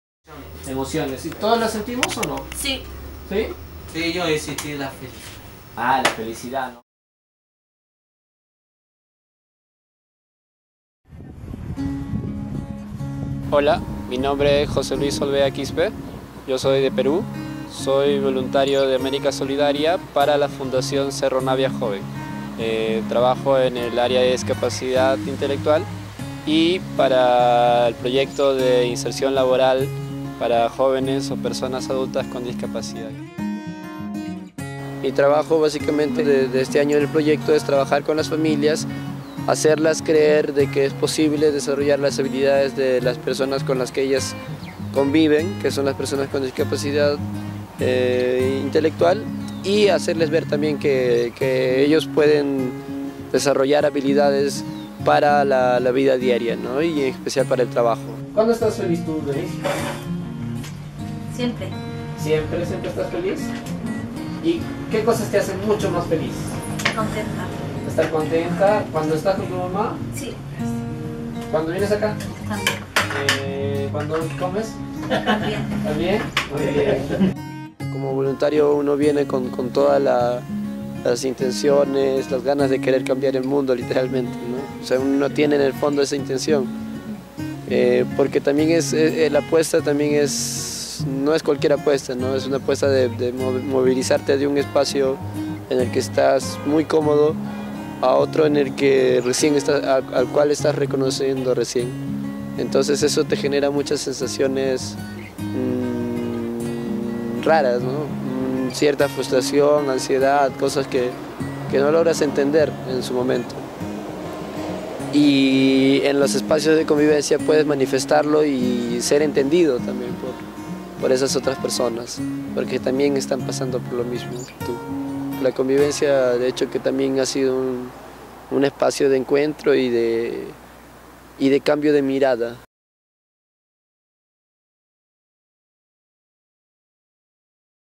Voluntarios, un año al sur del mundo - Capítulo 8
Conoce la historia de José Luis Olvea, profesional voluntario de América Solidaria de Perú, que llegó en marzo de 2016 a Chile para trabajar en el proyecto "Integración socio laboral para jóvenes y adultos en situación de discapacidad intelectual de la comuna de Cerro Navia". El cual desarrollamos en conjunto con la fundación Cerro Navia Joven.